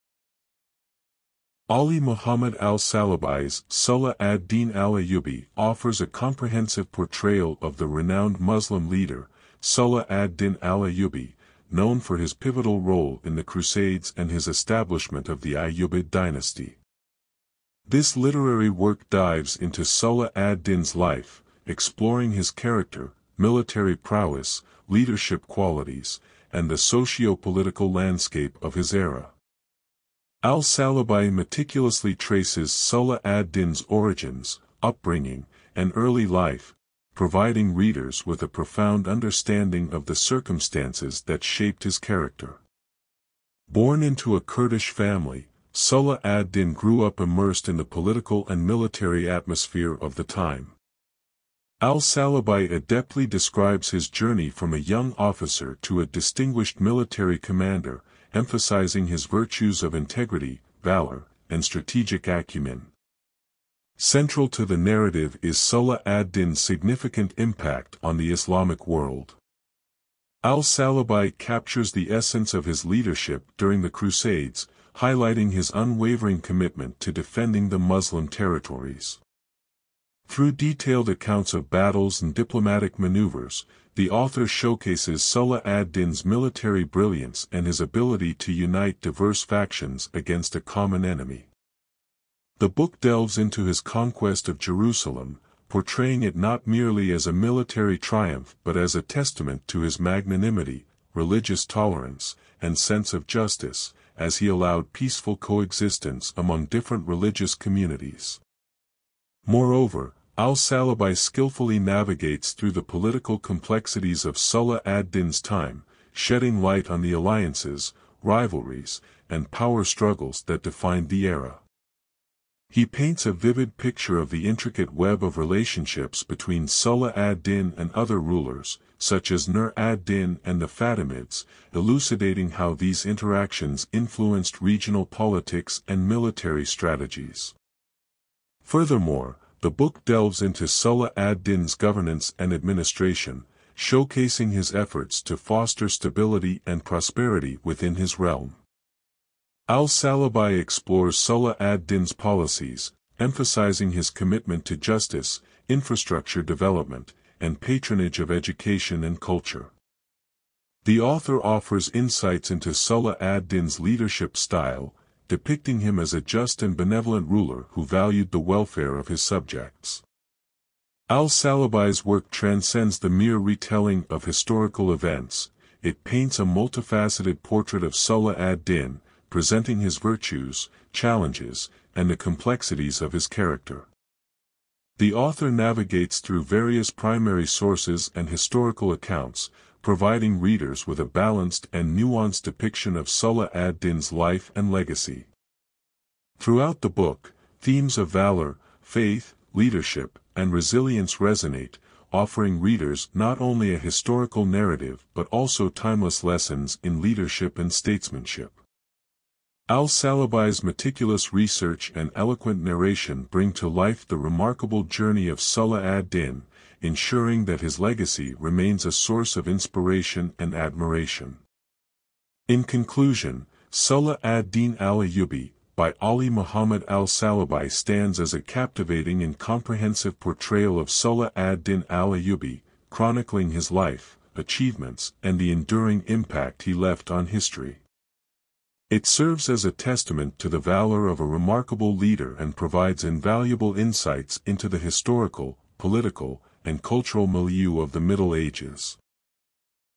"Salâh ad-Deen al-Ayubi" By علي محمد الصلابي
Ali Muhammad al-Sallabi’s "Salâh ad-Deen al-Ayubi" offers a comprehensive portrayal of the renowned Muslim leader, Salah ad-Din al-Ayubi, known for his pivotal role in the Crusades and his establishment of the Ayyubid dynasty. This literary work dives into Salah ad-Din's life, exploring his character, military prowess, leadership qualities, and the socio-political landscape of his era.Al-Sallabi meticulously traces Salah ad-Din's origins, upbringing, and early life, providing readers with a profound understanding of the circumstances that shaped his character. Born into a Kurdish family, Salah ad-Din grew up immersed in the political and military atmosphere of the time. Al-Sallabi adeptly describes his journey from a young officer to a distinguished military commander, emphasizing his virtues of integrity, valor, and strategic acumen.Central to the narrative is Salah ad-Din's significant impact on the Islamic world. Al-Sallabi captures the essence of his leadership during the Crusades, highlighting his unwavering commitment to defending the Muslim territories. Through detailed accounts of battles and diplomatic maneuvers, the author showcases Salah ad-Din's military brilliance and his ability to unite diverse factions against a common enemy. The book delves into his conquest of Jerusalem, portraying it not merely as a military triumph but as a testament to his magnanimity, religious tolerance, and sense of justice, as he allowed peaceful coexistence among different religious communities.Moreover, al-Sallabi skillfully navigates through the political complexities of Salah ad-Din's time, shedding light on the alliances, rivalries, and power struggles that defined the era. He paints a vivid picture of the intricate web of relationships between Salah ad-Din and other rulers, such as Nur ad-Din and the Fatimids, elucidating how these interactions influenced regional politics and military strategies.Furthermore, the book delves into Salah ad-Din's governance and administration, showcasing his efforts to foster stability and prosperity within his realm. Al-Sallabi explores Salah ad-Din's policies, emphasizing his commitment to justice, infrastructure development, and patronage of education and culture. The author offers insights into Salah ad-Din's leadership style, depicting him as a just and benevolent ruler who valued the welfare of his subjects.Al-Sallabi's work transcends the mere retelling of historical events; it paints a multifaceted portrait of Salah ad-Din, presenting his virtues, challenges, and the complexities of his character. The author navigates through various primary sources and historical accounts, providing readers with a balanced and nuanced depiction of Salah ad-Din's life and legacy.Throughout the book, themes of valor, faith, leadership, and resilience resonate, offering readers not only a historical narrative but also timeless lessons in leadership and statesmanship. Al-Sallabi’s meticulous research and eloquent narration bring to life the remarkable journey of Salah ad-Din, ensuring that his legacy remains a source of inspiration and admiration.In conclusion, "Salâh ad-Deen al-Ayubi" by Ali Muhammad al-Sallabi stands as a captivating and comprehensive portrayal of Salah ad-Din al-Ayubi, chronicling his life, achievements, and the enduring impact he left on history. It serves as a testament to the valor of a remarkable leader and provides invaluable insights into the historical, political, and cultural milieu of the Middle Ages.